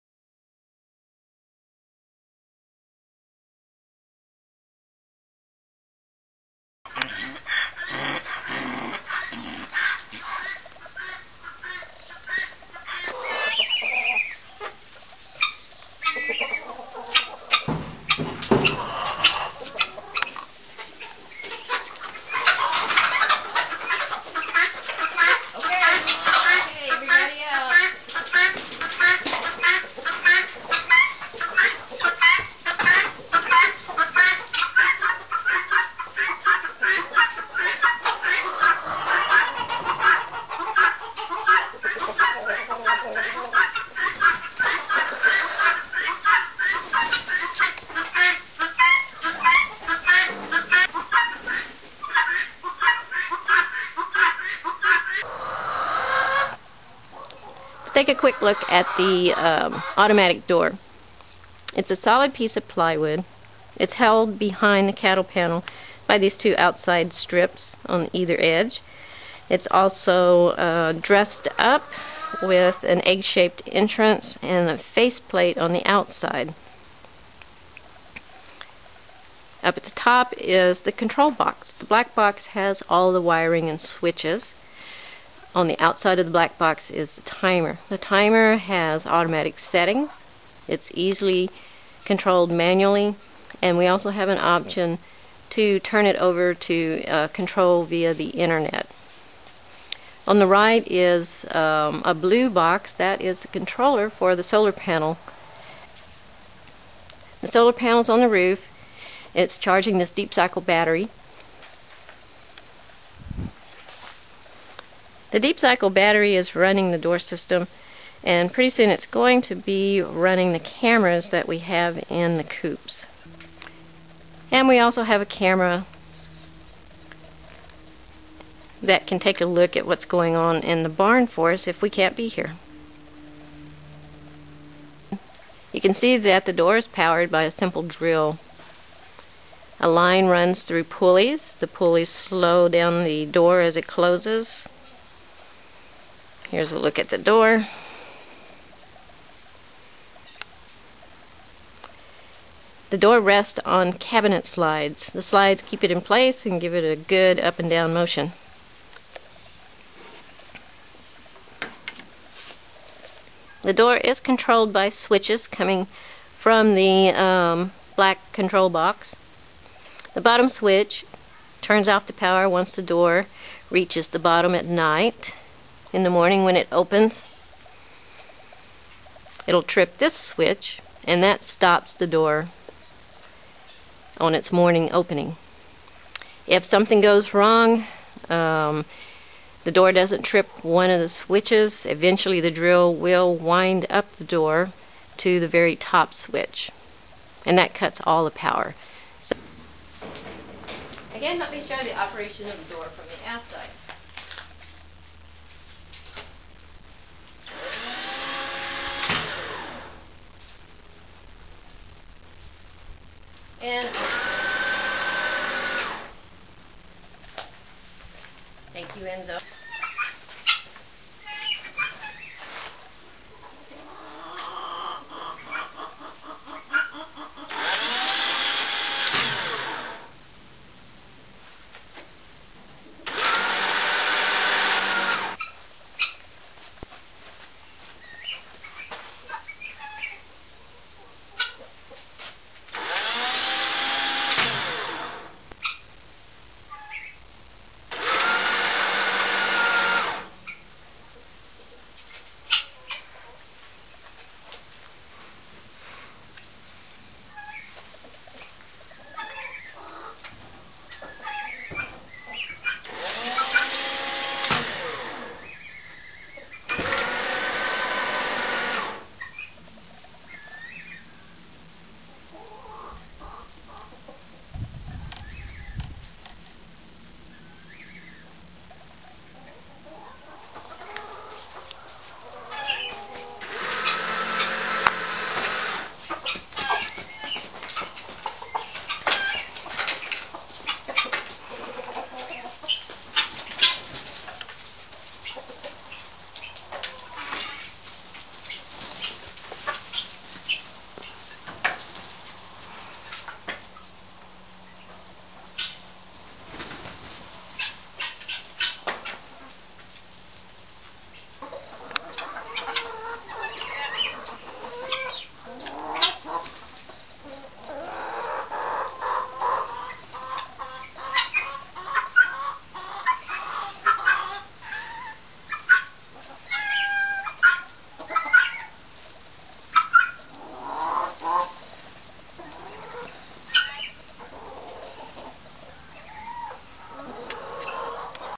Automatic Chicken Coop Door by Penny
Our solar powered automatic door is a thumbs up with our chickens.  They are released early in the morning while we rest or do other things, and they are shut in safely at night.  We can control the door and see inside the coop with our web cam from the house or from any location, over the internet.  But we prefer to set the timer to open and close the coop door automatically.

My husband did the electronics

There is no design to share, I'm sorry, my husband did the electronics but has not had the time to scketch it all out. But here is how it works and hopefully you can make a similar switch box from my description.

We don't have AC power in the barn, that's why we got interested in solar charging a marine deep cycle battery (that we already had) to run the door. The coop door doesn't use much power since it only works twice a day. So a smaller battery could work -- batteries that power automatic gates, etc. We have sun year round. It is rare for clouds to hide the sun more than 4-5 days.

Battery powered drills worked well for us, it is shaped right and is strong, and usually someone has an old drill they cant use because the original batteries have failed.

We opened the drill case and soldered our wires directly to the drill motor. Their were too many problems trying to work through the drills trigger -- it overheated, etc.

You will need two double pull, double throw relays. The first relay is to reverse the polarity of the wiring and tell the drill to move up or down. The second relay is used to trip the trigger switches on top and bottom. Wire the relays so when the door rises the top trigger switch cuts the power to the drill. And when the two relays are energised then the drill is reversed and the bottom trigger switch is active and shuts off the drill when the door closes.

When wired correctly, the door will rise when the wires are in the open position and go down when in the closed position. We used a 12v DC timer to open and close the door.
"Flex Charge" part number PRG TMR 12V.

Look online or at a local do-it-yourself electronics/computer parts store for large switches that are easier to work with.

Please note the double pull double throw switches need to be rated for 12v to energize the relays correctly.

Also, If the voltage drops below 12V, the relays might not work correctly -- such as your battery is too weak. The drill will work, but door movement won't function right. So be sure when you are testing and trying out your design, your voltage is strong.

The door panel itself can be made to suit your situation. We used cabinet slides to mount the lift door onto the panel (Home Depot). The plywood has to be free of warp or the door will bind. But there are other ways to do it.

We looked at quite a few of the door designs on UTube and that is where we got the ideal of using a DC drill.

Our door is not predator proof, we have 4 dogs and donkeys charged to take care of coop security. Keep in mind that if a racoon were to lift the door enough to "release" the bottom switch, the drill would be activated and lift the door -- so, your design might need a locking mechanism. There is a clever passive design on UTube that uses wood blocks that drop down into notches as the door closes to keep racoons from lifting the door.

One last thing, the door is kinda heavy, so to avoid accidents with the chickens, we set it to close well after the chickens have chosen their roosting spot for the nite.